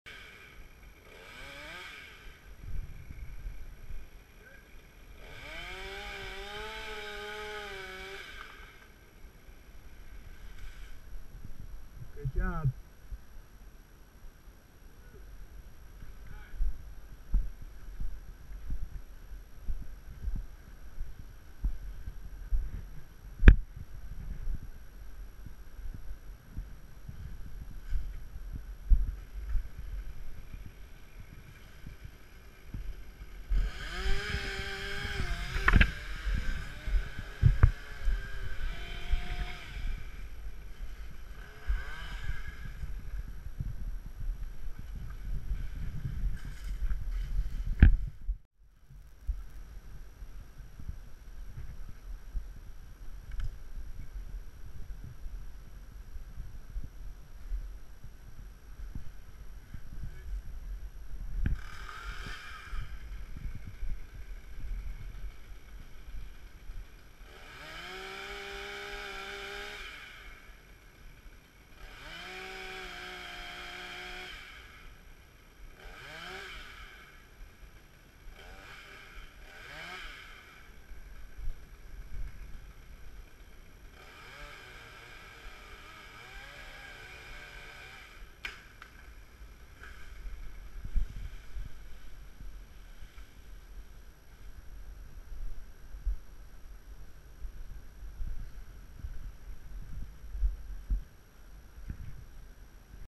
Lowering limbs over roof with a mini port a wrap
I like the mini for smaller limbs because it is compact, lightweight, and easy for one ground worker to install on the tree.